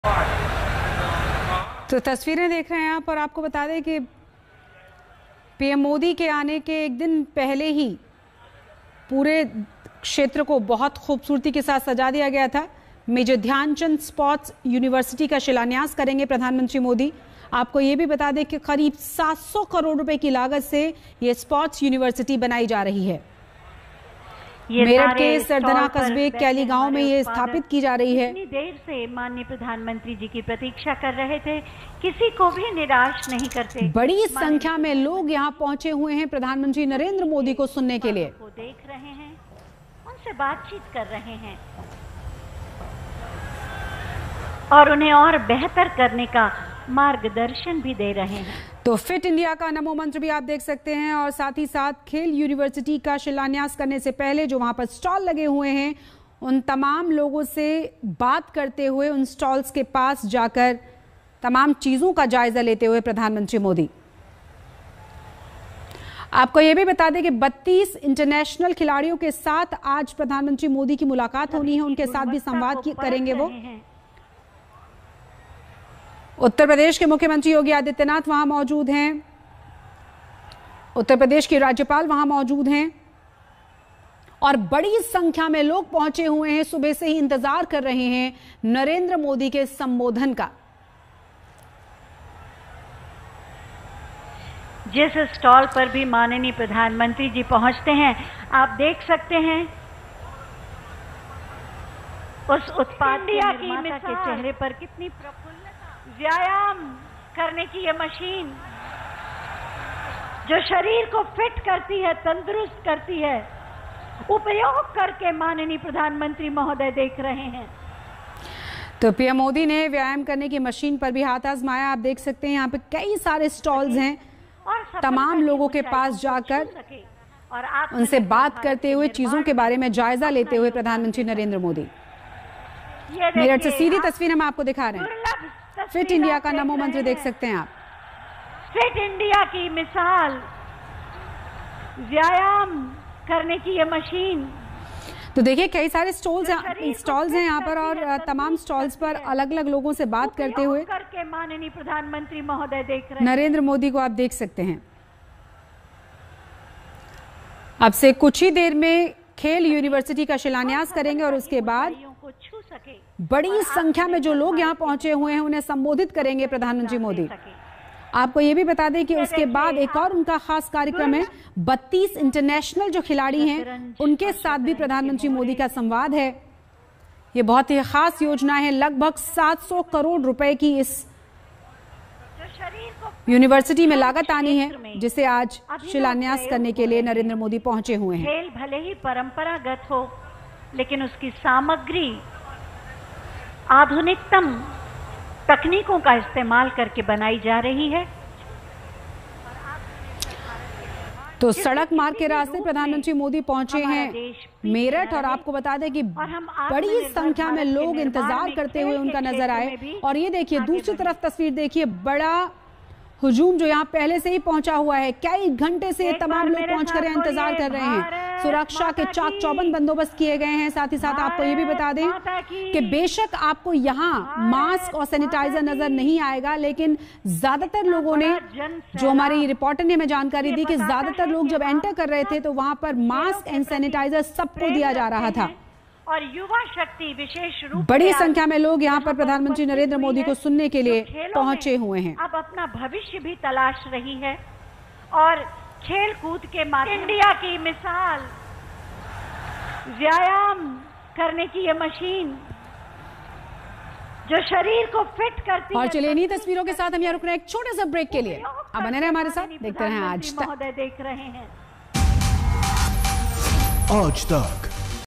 UP Election 2022: PM Modi ने दिया Fit India का मंत्र, व्यायाम मशीन पर आजमाया हाथ ! Latest News
UP Election 2022 के मद्देनजर PM Modi आज Meerut दौरे पर हैं. PM Modi आज Dhyanchand Sports University का शिलान्यास करेंगे. लेकिन उससे पहले PM Modi स्पोर्ट्स कॉप्लेक्स में व्यायाम मशीन पर हाथ आजमाते दिखें. देखिए खास वीडियो.